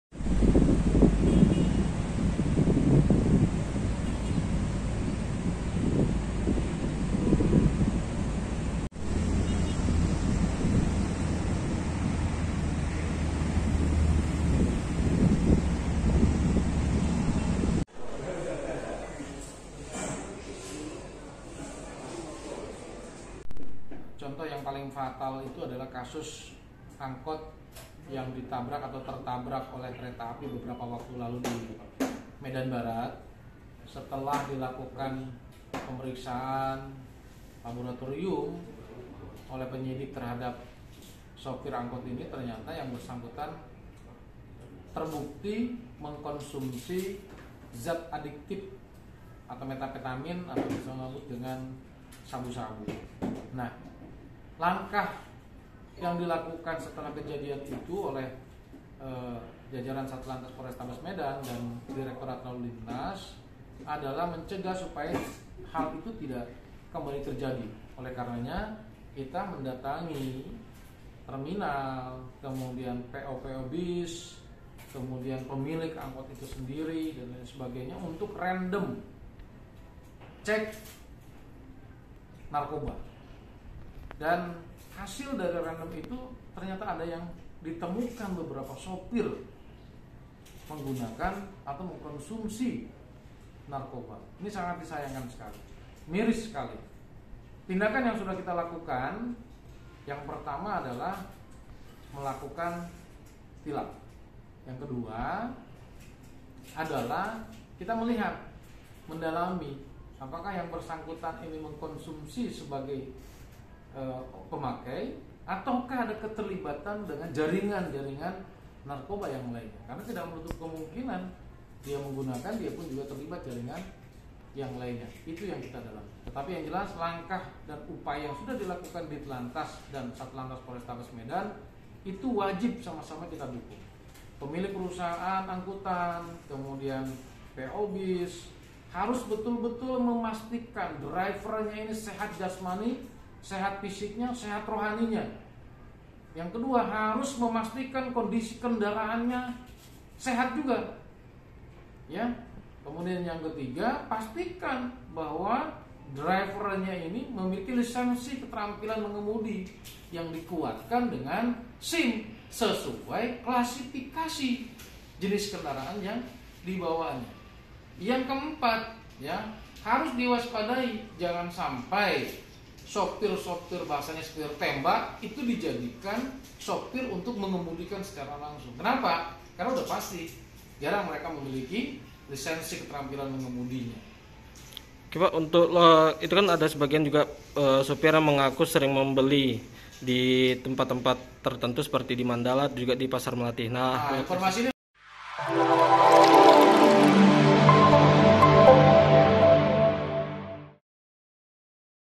Penjelasan Polda Sumut Terkait Sopir Angkot Terjaring Razia Positif Narkoba
TRIBUN-MEDAN.COM, MEDAN - Puluhan sopir angkot positif mengkonsumsi narkoba setelah terjaring razia yang dilakukan oleh petugas gabungan di sejumlah titik wilayah Kota Medan.

Para sopir tersebut diamankan ke Pos Lantas Polrestabes Medan untuk menjalani pemeriksaan lebih lanjut turut bersama mobilnya.

Kabid Humas Polda Sumut, Kombes Pol Hadi Wahyudi mengungkapkan pihaknya saat ini masih melakukan pemeriksaan lebih lanjut mengenai status daripada sang sopir.

"Masih kita periksa, apakah ada keterlibatan jaringan atau sebagainya," ujarnya kepada Tribun Medan, Senin (20/12/2021).

Hadi menjelaskan, apabila para sopir tersebut hanya sebagai pemakai, maka akan dilakukan rehabilitasi.

Terkait lokasi tempat para sang supir mendapatkan barang haram tersebut, dirinya menegaskan untuk segera melaporkan kepada pihak kepolisian.

"Laporkan kepada Polisi dimana tempatnya, akan kita tindak lanjuti.," tegasnya.

Hadi pun meminta agar para pemilik angkutan umum untuk lebih memperhatikan sopir angkot, apalagi saat ini kebanyakan sopir tersebut merupakan sopir tembak.

"Jangan sampai, sopir tembak itu dijadikan sopir untuk mengemudi. Karena mereka sudah pasti tidak memiliki lisensi untuk mengemudi," tandasnya.